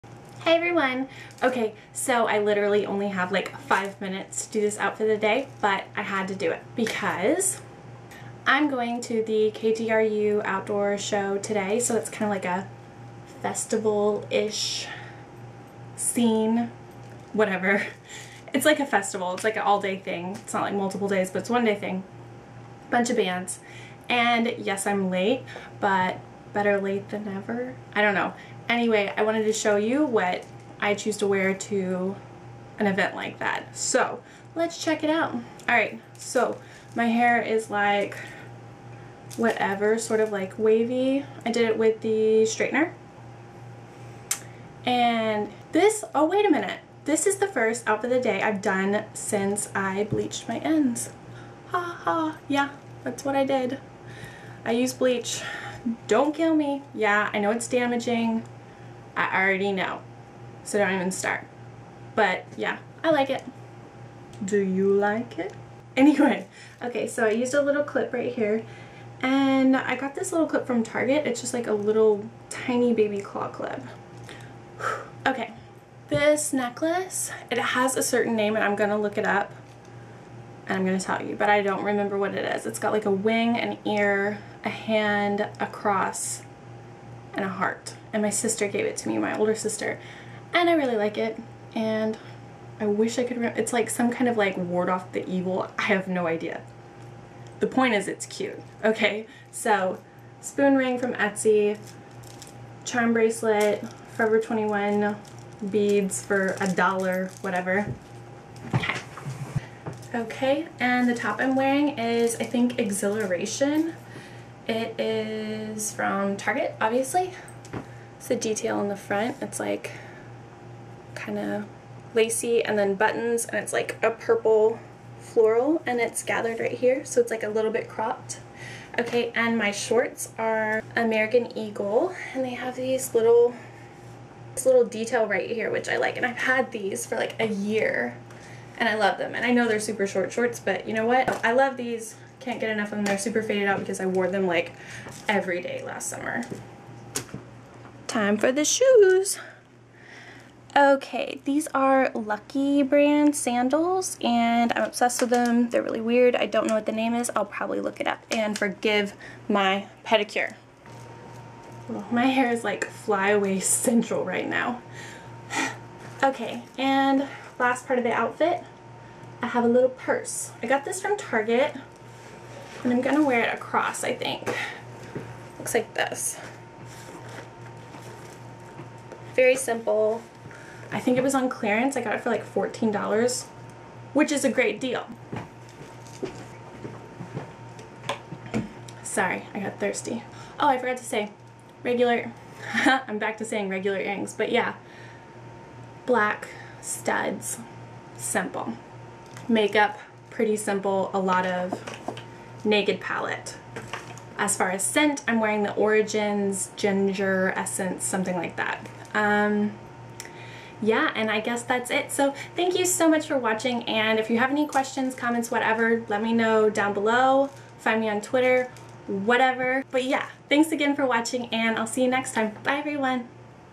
OOTD: Festival Ready for the KTRU Outdoor Show | LeighAnnSays
Here is a tiny outfit of the day video I filmed before going to the KTRU outdoor show. It's a simple look and I was mainly focused on staying cool and comfortable.

xo Leigh Ann